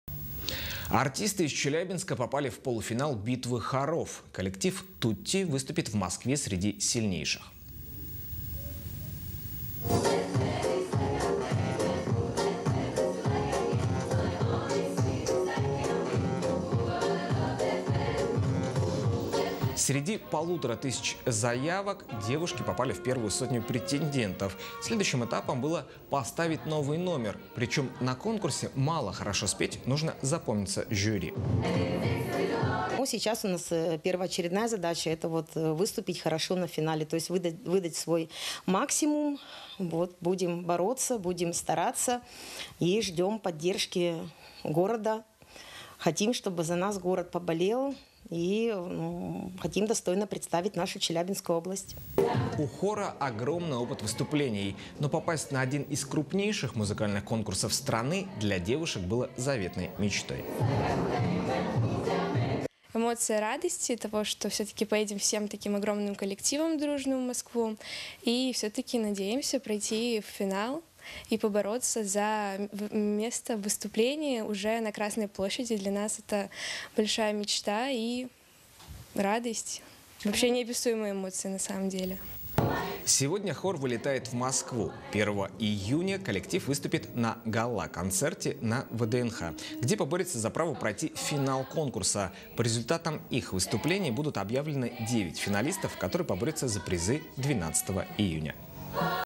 Челябинский коллектив "Тутти" попал в полуфинал «Битвы хоров»
Коллектив «Тутти» выступит в Москве среди сильнейших. Среди полутора тысячи заявок девушки попали в первую сотню претендентов.